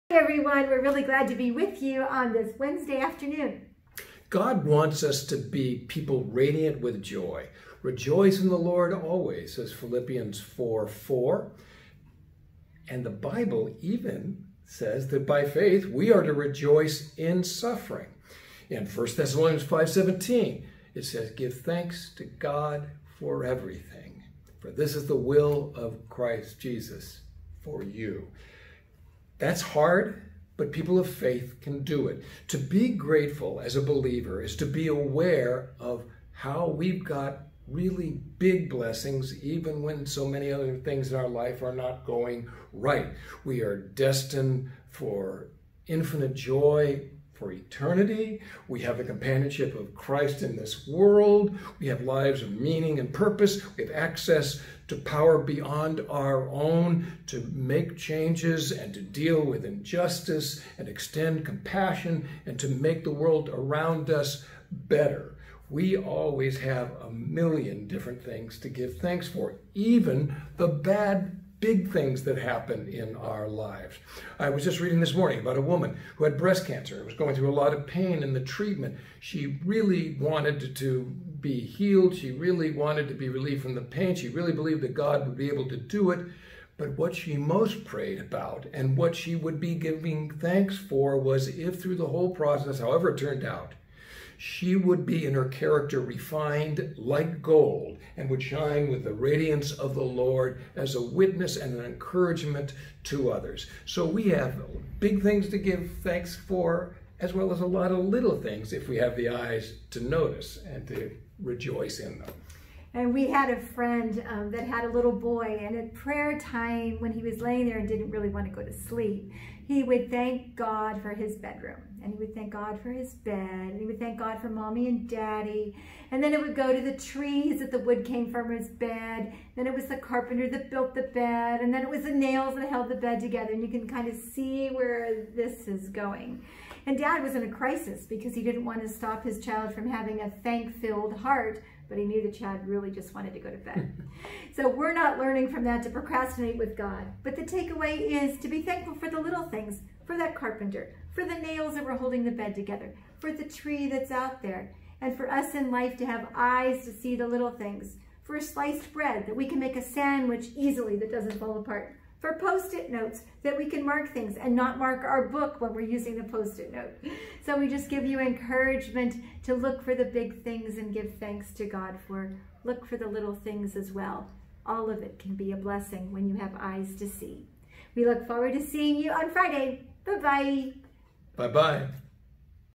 Words from the Pastors - Wednesday, September 2, 2020
Words from the Pastors: Blessings, big & small.

Pastor Rob and Pastor Adele are thinking of you and praying for you at Covenant Presbyterian Church in Long Beach!